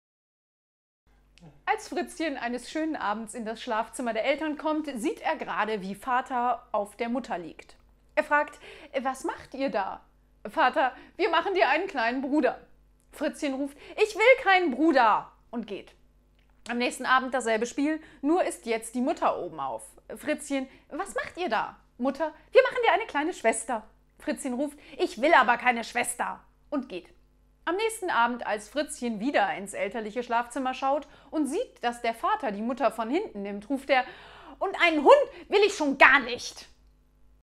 Weder Bruder, Schwester noch Hund
Vorgetragen von Frau Freyberg

Als Fritzchen eines schönen Abends in das Schlafzimmer der Eltern kommt, sieht er gerade, wie Vater auf der Mutter liegt. Er fragt: "Was macht ihr da?" Vater: "Wir machen dir einen kleinen Bruder!" Fritzchen ruft: "Ich will keinen Bruder!" und geht. Am nächsten Abend dasselbe Spiel, nur ist jetzt die Mutter obenauf. Fritzchen: "Was macht ihr da?" Mutter: "Wir machen dir eine kleine Schwester!" Fritzchen ruft: "Ich will aber keine Schwester!" und geht. Am nächsten Abend, als Fritzchen wieder ins elterliche Schlafzimmer schaut und sieht, dass der Vater die Mutter von hinten nimmt, ruft er: "Und einen Hund will ich schon gar nicht!"